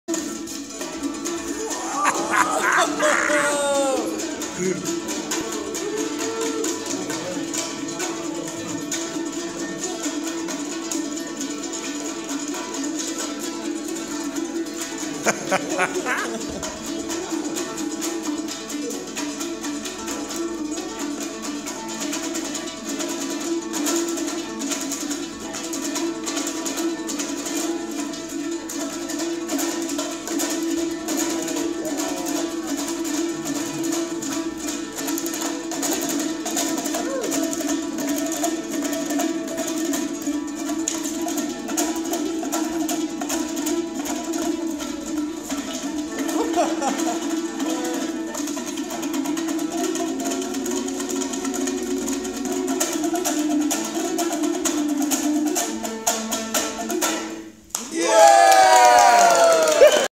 Kyrgyz Komuz demonstration
Ordo Sakhna demonstrates the Komuz to the Henhouse Prowlers during their first meeting in Bishkek.